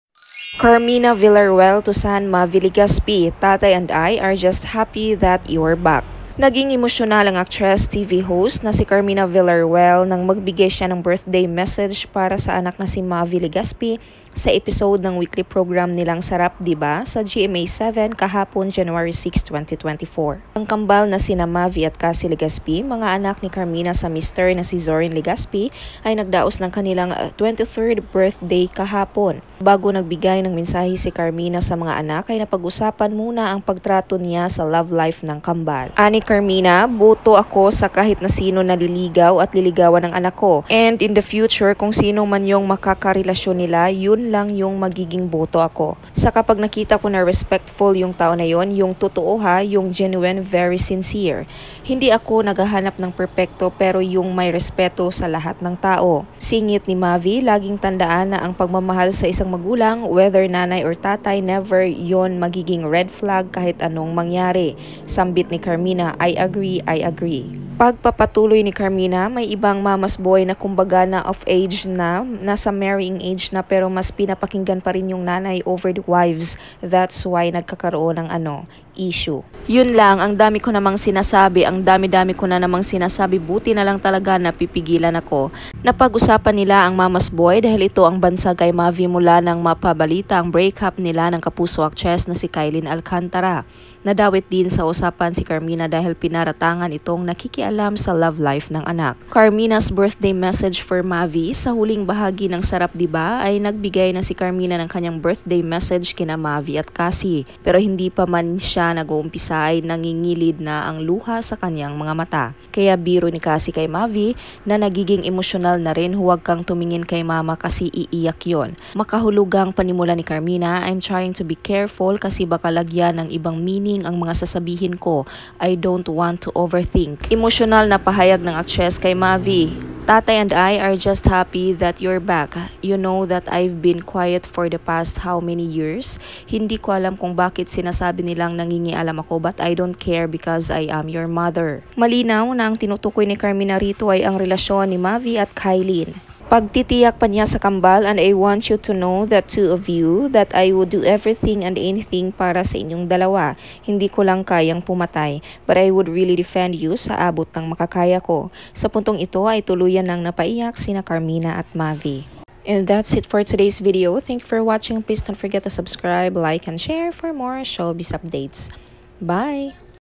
Carmina Villarroel to son Mavy Legaspi: “Tatay and I are just happy that you're back.”
Carmina Villarroel clarifies she does not meddle in her children's love life.

Ang kambal na sina Mavy at Cassy Legaspi, mga anak ni Carmina sa mister na si Zoren Legaspi, ay nagdaos ng kanilang 23rd birthday kahapon.

Bago nagbigay ng mensahe si Carmina sa mga anak ay napag-usapan muna ang pagtrato niya sa love life ng kambal.

Ani Carmina, “Boto ako sa kahit na sino na liligaw at liligawan ng anak ko.

"And in the future, kung sino man yung makakarelasyon nila, yun lang yung magiging boto ako.